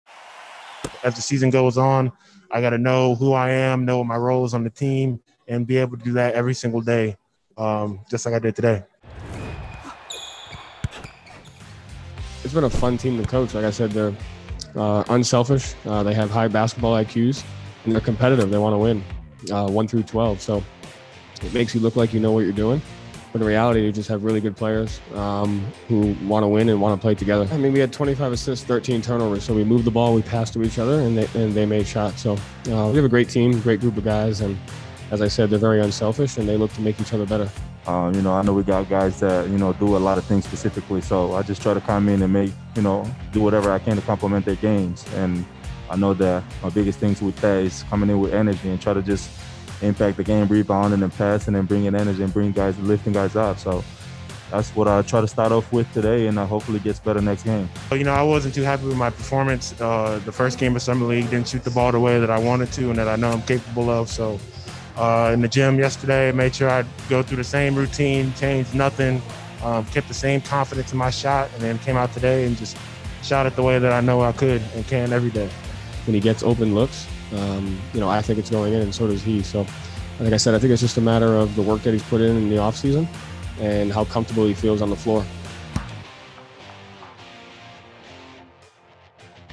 8/10 Putnam Postgame Report: Making Each Other Better
The Celtics rolled to a 107-82 victory lead by Aaron Nesmith's 33 points.